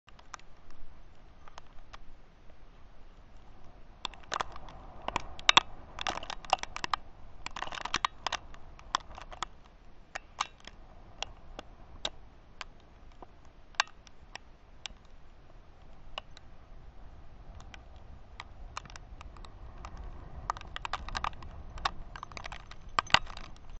SNOW STORM WASHINGTON HEIGHTS,NYC. Feb,8,2013. Snow Vlog 1
Go Pro Hero 3.  Bennet Park, Washington Heights.  Winter Storm- 2:35pm.  Straight from the camera.
The sound is not that bad.